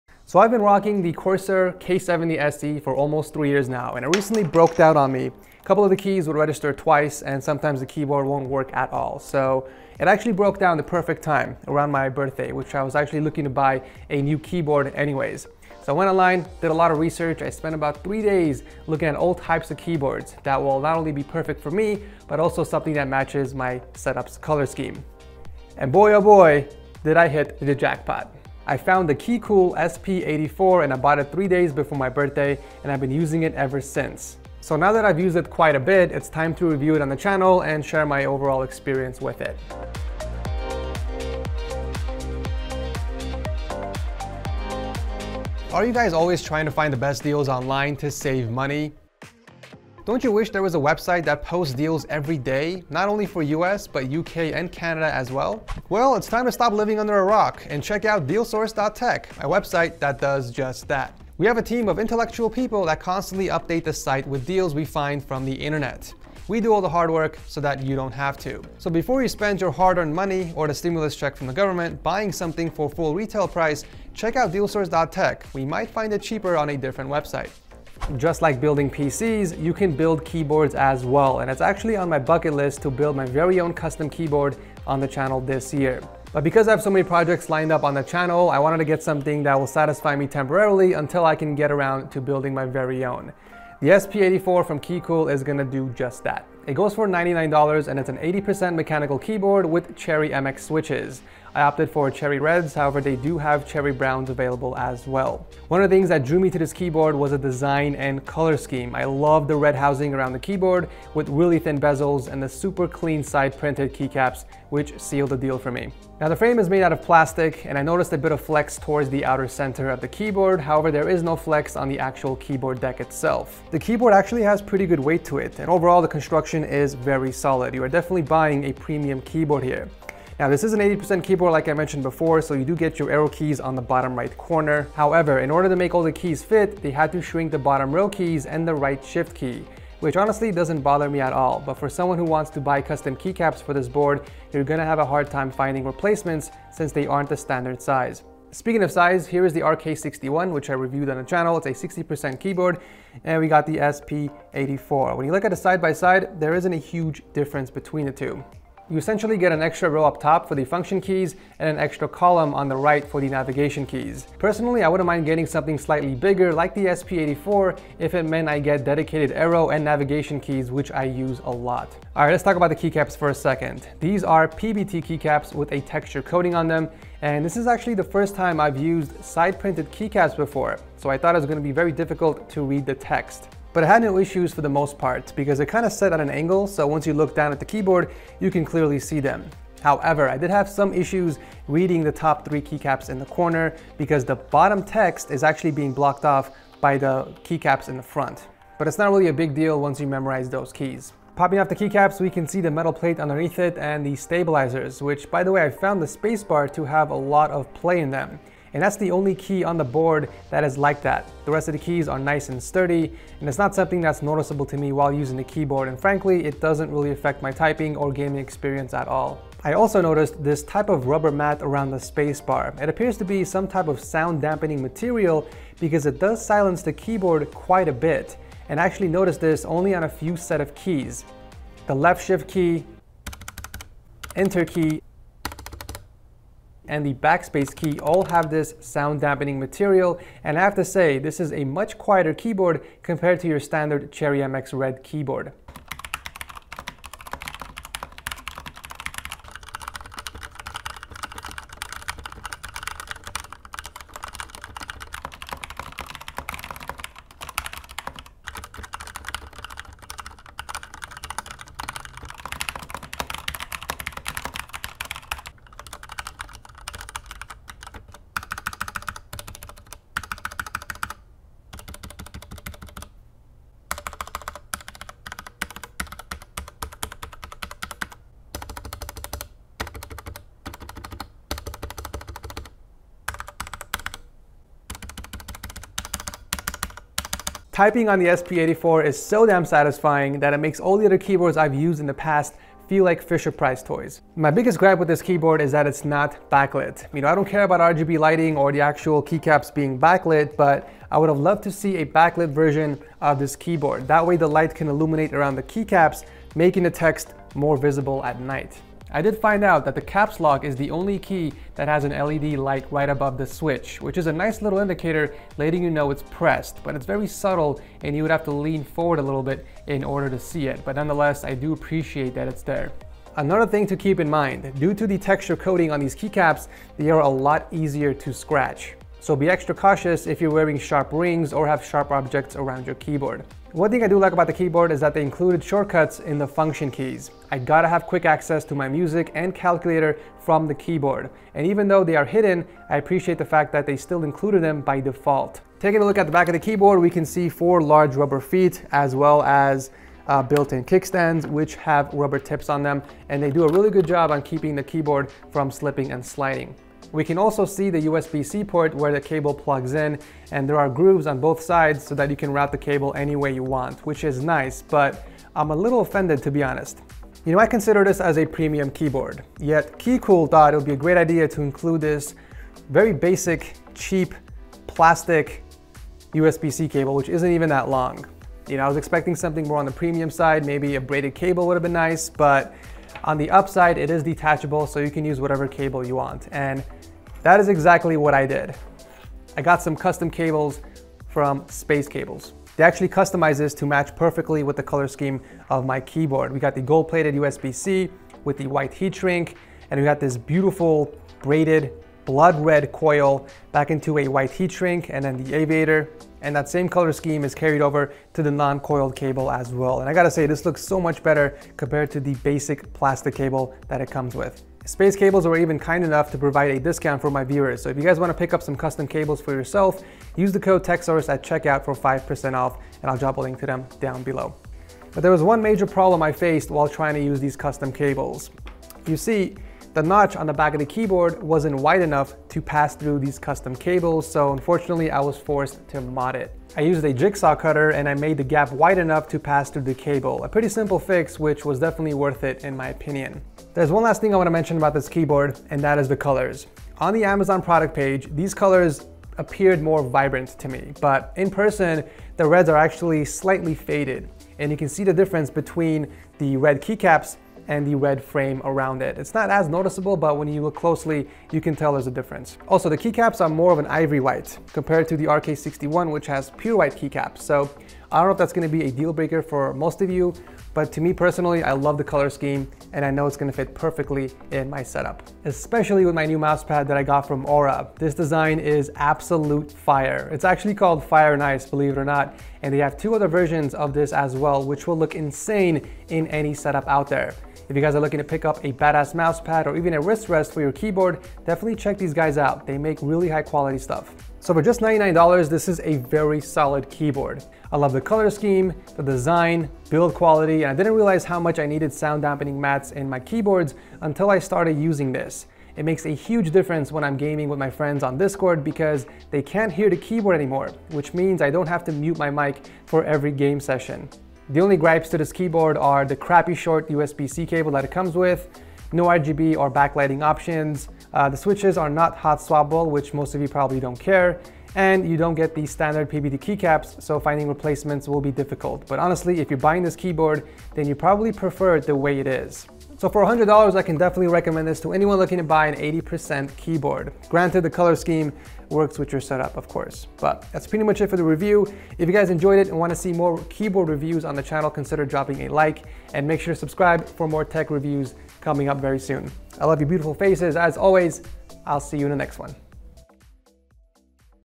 My New Gaming Keyboard is Badass - Keycool SP84 Review
Before I build my very own custom keyboard, I thought I would buy myself something to keep me satisfied until then.